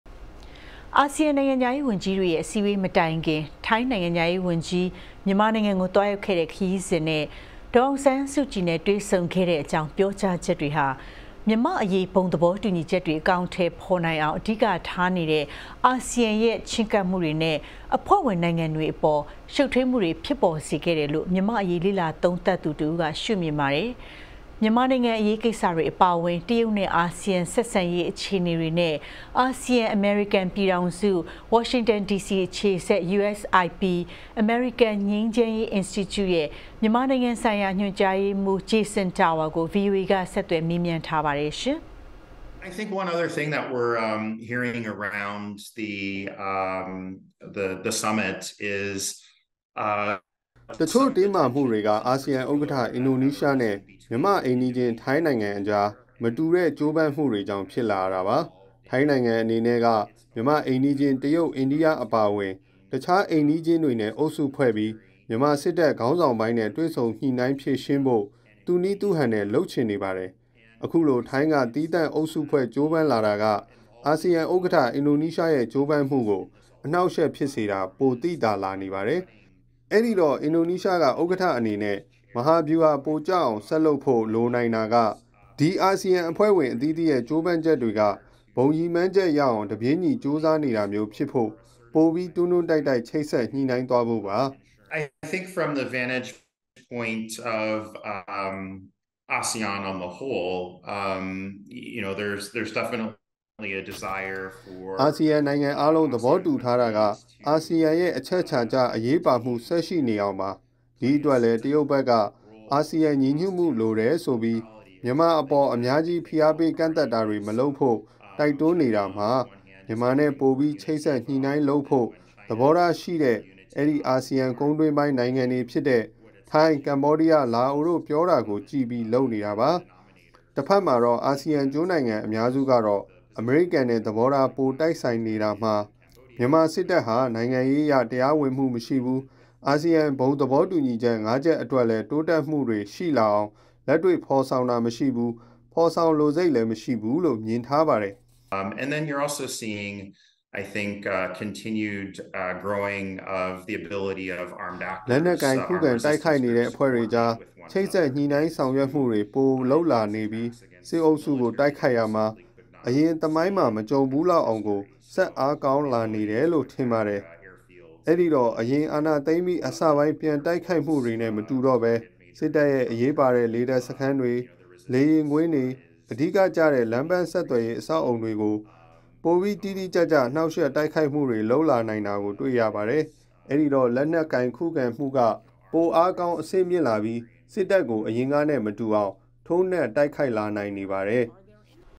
မြန်မာ့အရေး ထိုင်းနဲ့ အာဆီယံချဉ်းကပ်ပုံ ကွဲလွဲ
မြန်မာနိုင်ငံအရေးကိစ္စတွေ အပါအဝင်၊ တရုတ်နဲ့ အာဆီယံ အရှေ့တောင်အာရှနိုင်ငံများအသင်း ဆက်ဆံရေး အခြေအနေတွေ ကို ဝါရှင်တန်ဒီစီအခြေစိုက် USIP အမေရိကန်ငြိမ်းချမ်းရေး အင်စတီကျူရဲ့ မြန်မာနိုင်ငံဆိုင်ရာ ညွန်ကြားရေးမှူး Jason Tower ကို VOA က ဆက်သွယ်မေးမြန်းထားပါတယ်။